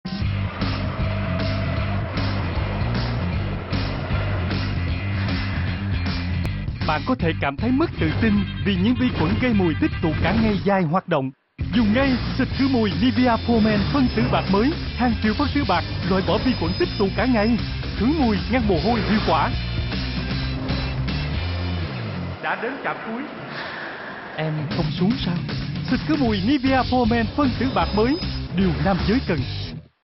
Nivea For Men Deodorant Silver Protect TVC - Vietnam
New TV Commercial in Vietnam from Nivea For Men Deodorant Roll On Silver Protection